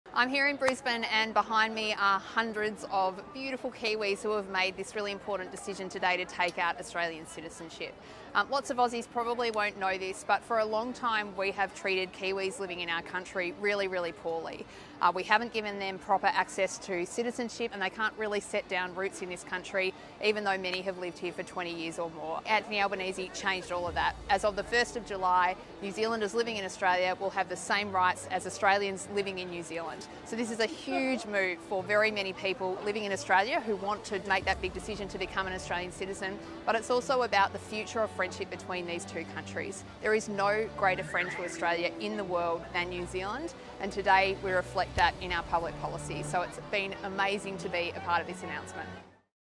A direct pathway to Australian citizenship for New Zealanders
There’s no greater friend to Australia than New Zealand. In fact, we’re family.

That’s why from July 1, Albanese Labor Government is strengthening our ties and providing a direct pathway to Australian citizenship for New Zealanders.